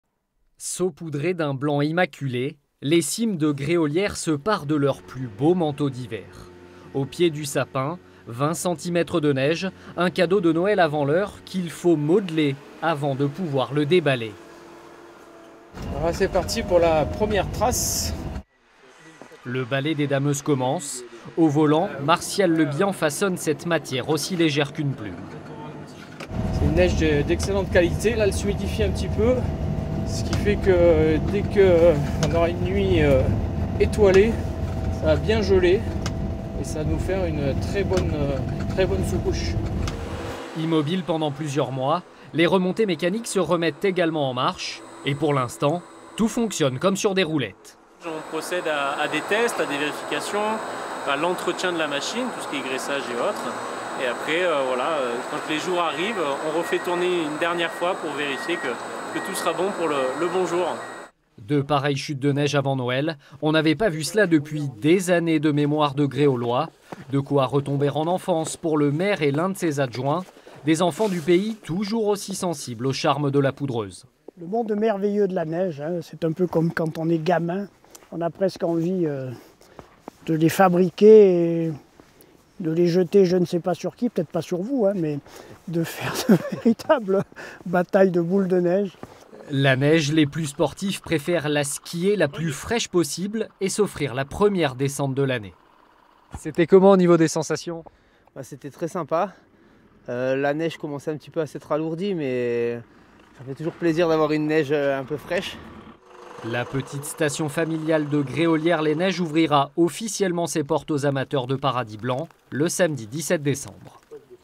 Gréolières les Neiges : la neige au rendez-vous pour l'ouverture, après deux saisons difficiles
Dans cette petite commune des Alpes-Maritimes, à Gréolières, la neige revient en masse. Après une saison 2020/2021 avec un record d'enneigement mais compliquée en raison du Covid et une saison 2021/2022 avec très peu de neige, c'est une excellente nouvelle pour tous les professionnels et amateurs de sports d'hiver à l'approche des vacances de Noël. L'heure est donc aux préparatifs pour accueillir les visiteurs dans les meilleures conditions dès l'ouverture ce samedi 17 décembre.